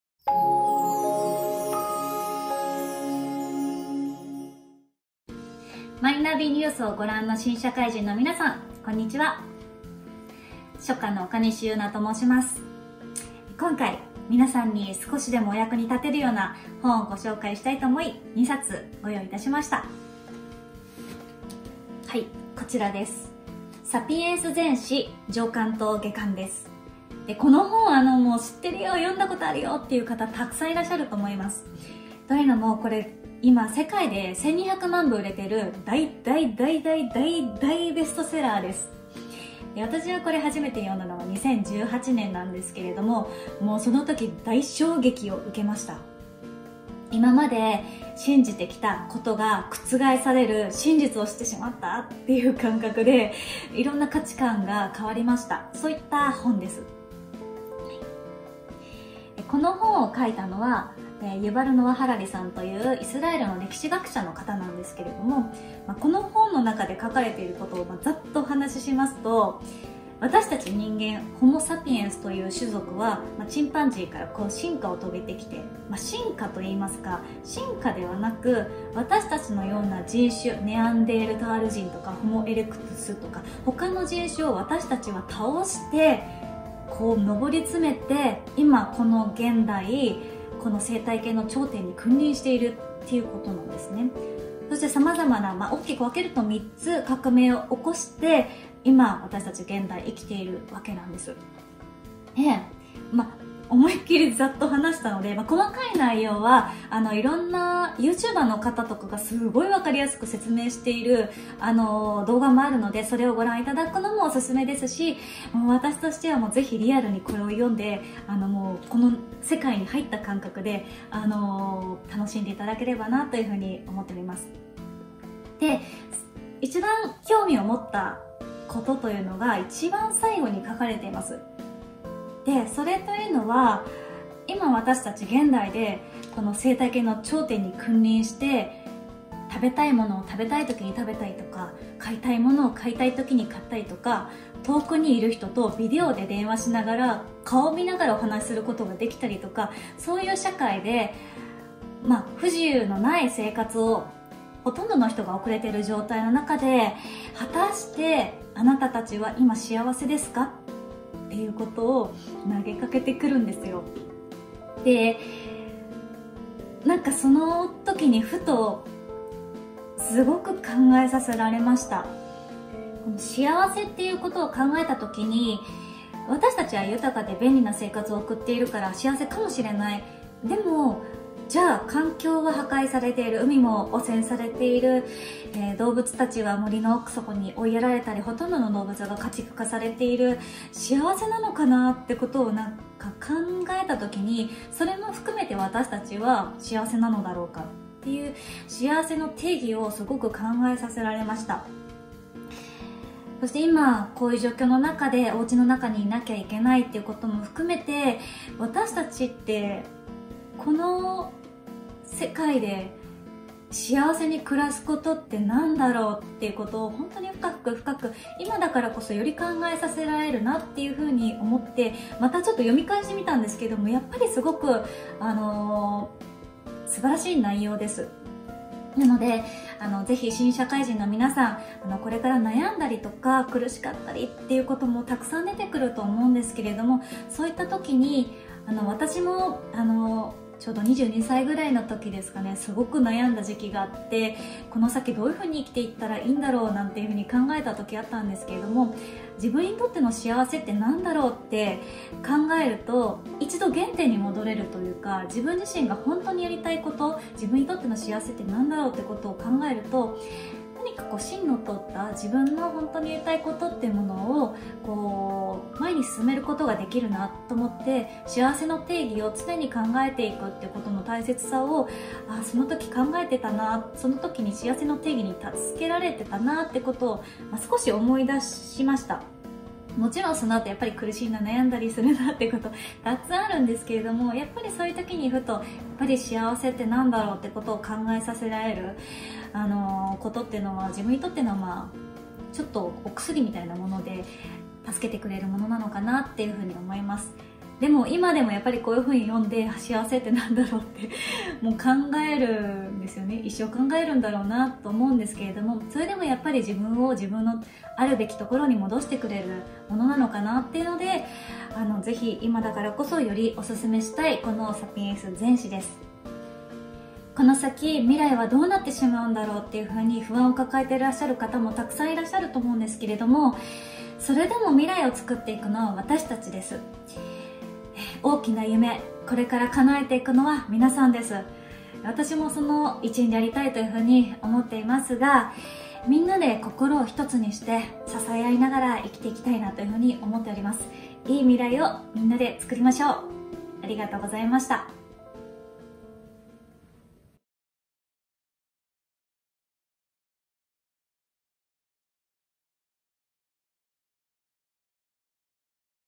ONEBOOK -岡西 佑奈(書家/書道家/アーティスト)-
新型コロナウイルスの影響で、不安の門出を迎えることになってしまった
新社会人の方々に向けて、各界の著名人がセレクトした1冊をお届けしていく
「ONEBOOK-私に影響を与えた1冊-」。
今回登場いただいたのは、書家/書道家/アーティストの岡西佑奈さん。メディアとして、応援のメッセージと共に少しでも今の時間にとってプラスになる情報・気持ちをお伝えできればと思います。

■Guest
岡西 佑奈
書家/書道家/アーティスト。高校在学中に師範免許を取得し、これまでに受賞実績多数。近年では、墨象や「青曲」「真言」など絵画も手がけ、国内外に精力的に出展。現在、世界から集まった希望の言葉を書にする「希望の書」を自身のSNSで始め、共同通信でも連載配信がスタート。作品集「線の美」(青幻舎)を出版。

■Book
サピエンス全史 /ユヴァル・ノア・ハラリ  (著), 柴田裕之 (翻訳)/ 河出書房新社

本プロジェクトは、有志でご協力をいただいた方々のメッセージをお届けしています。